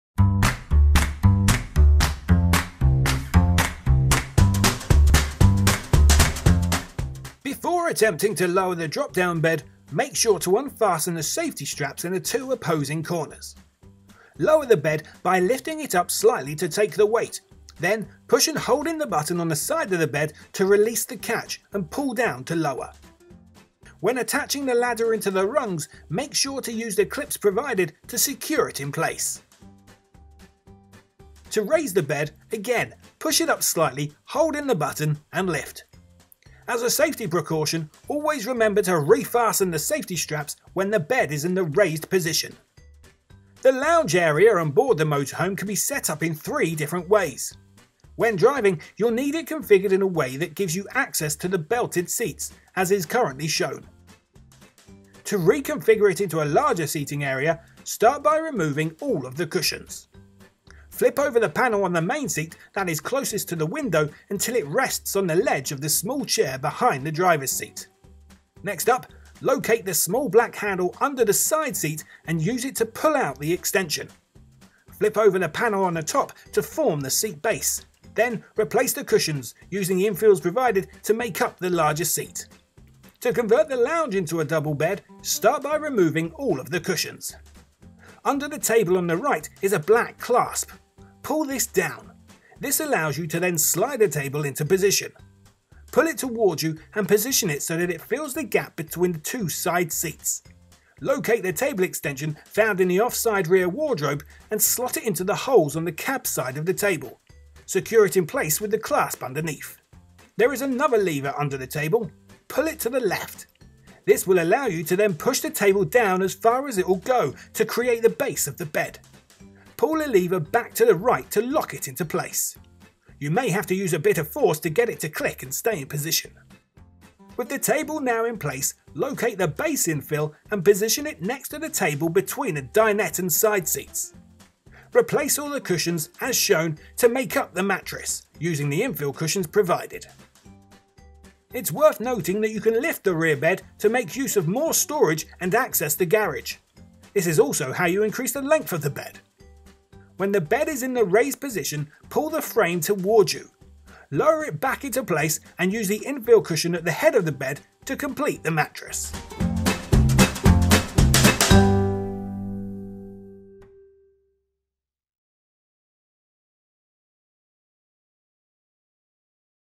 How to Set the Bed Up in a Motorhome
How to operate the drop-down bed and convert the dinette into a bed in a motorhome.

If you're struggling to find out to set the bed up in a motorhome, we've created this guide to help you. We show you how to convert the area of a motorhome into a double bed.